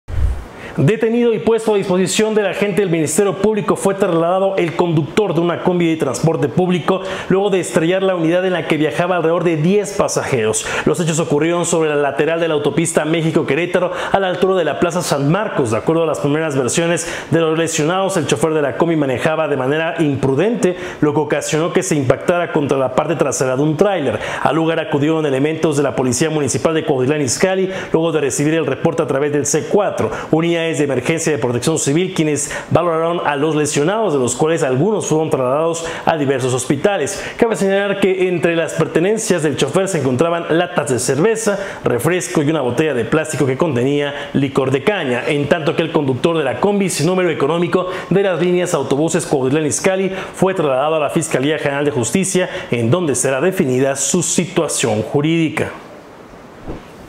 Detienen a conductor de transporte público que se impactó contra un tráiler y deja lesionados
Detenido y puesto a disposición del agente del Ministerio Público fue trasladado el conductor de una combi de transporte público, luego de estrellar la unidad en la que viajaban alrededor de 10 pasajeros.

Los hechos ocurrieron sobre la lateral de la autopista México-Querétaro a la altura de plaza San Marcos, de acuerdo a las primeras versiones de los lesionados, el chofer de la combi manejaba de manera imprudente lo que ocasionó que se impactara contra la parte trasera de un tráiler.

Al lugar acudieron elementos de la policía municipal luego de recibir el reporte a través del C4, unidades de emergencias de Protección Civil quienes valoraron a los lesionados de los cuales algunos fueron trasladados a diversos hospitales.

Cabe  señalar que entre las pertenencias del chofer se encontraban latas de cerveza, refresco y una botella de plástico que contenía licor de caña.

En tanto que el conductor de la combi sin número económico de la línea Autobuses Cuautitlán Izcalli, fue trasladado a la Fiscalía General de Justicia en donde será definida su situación jurídica.